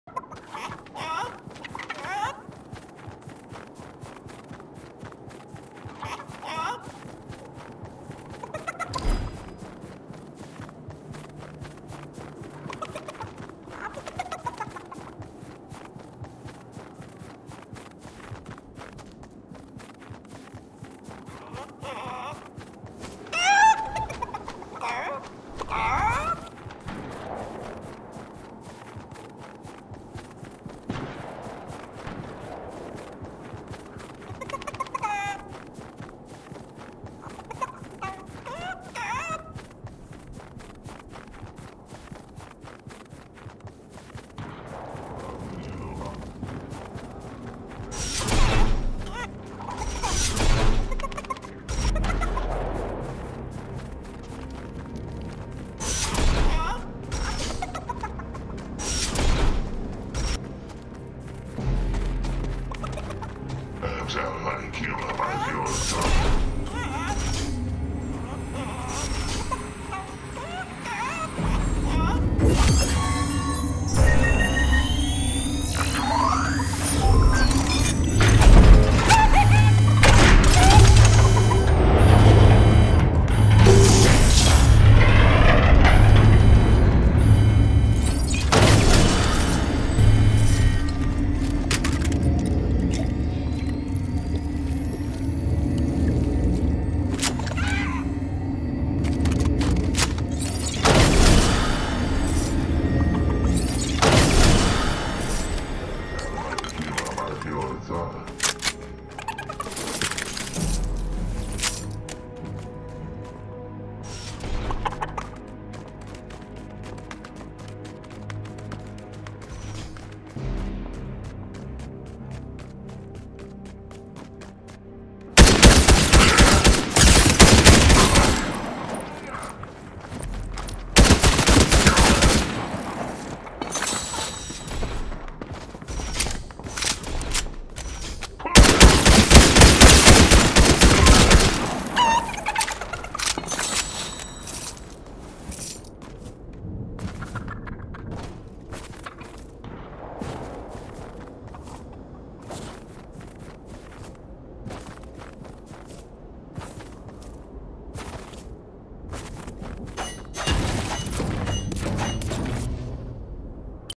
Chapter 3 Tresor öffnen ohne SCHLÜSSEL- mit Huhn öffnen - KEIN FAKE GEHT!
►Zocken bis der Arzt kommt ; das ist mein Motto.

Ich öffne den Tresor mit einem Huhn. Kein FAKE das GEHT!